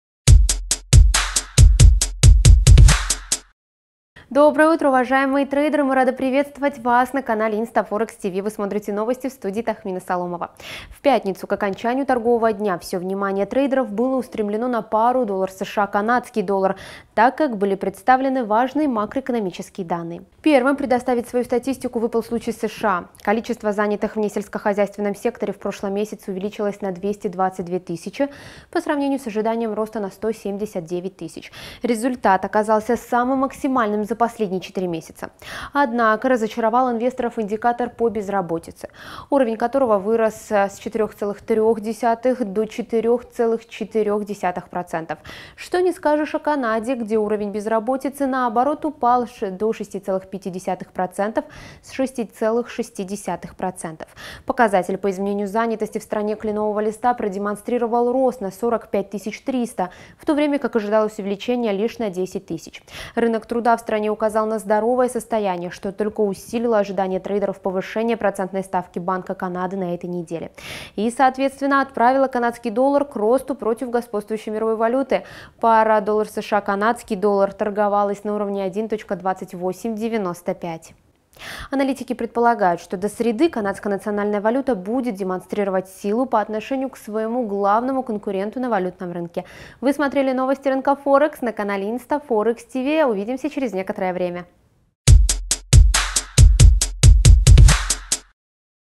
Доллару США не удалось вернуться к максимумам (10.07.2017)
В пятницу к окончанию торгового дня, все внимание трейдеров было устремлено на пару USD/CAD, так как были представлены важные макроэкономические данные.
Первым предоставить свою статистику выпал случай США. Количество занятых в несельскохозяйственном секторе в прошлом месяце увеличилось на 222 000 по сравнению с ожиданием роста на 179 000. Результат оказался самым максимальным за последние 4 месяца.
Однако разочаровал инвесторов индикатор по безработице, уровень которого в июне вырос с 4,3% до 4,4%.
Что не скажешь о Канаде, где уровень безработицы наоборот упал до 6,5% с 6,6%.
Показатель по изменению занятости в стране кленового листа продемонстрировал рост на 45 300, в то время как ожидалось увеличение лишь на 10 000. Рынок труда в стране указал на здоровое состояние, что только усилило ожидания трейдеров повышения процентной ставки Банка Канады на этой неделе.
И, соответветственно, отправило канадский доллар к росту против господствующей мировой валюты. Пара USD/CAD торговалась на уровне 1.2895.
Аналитики предполагают, что до среды канадская национальная валюта будет демонстрировать силу по отношению к своему главному конкуренту на валютном рынке.